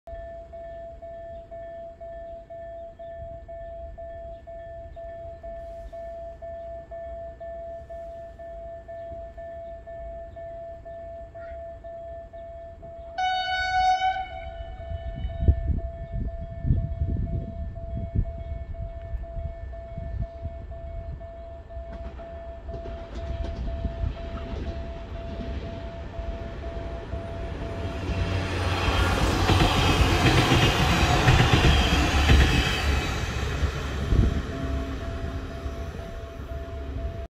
Pesa 730ML- 003 with a passenger train
Lithuanian railways (LTG- Lietuvos geležinkeliai) with a passenger train Pesa 730ML- 003 passing Jonava railroad crossing. Lithuania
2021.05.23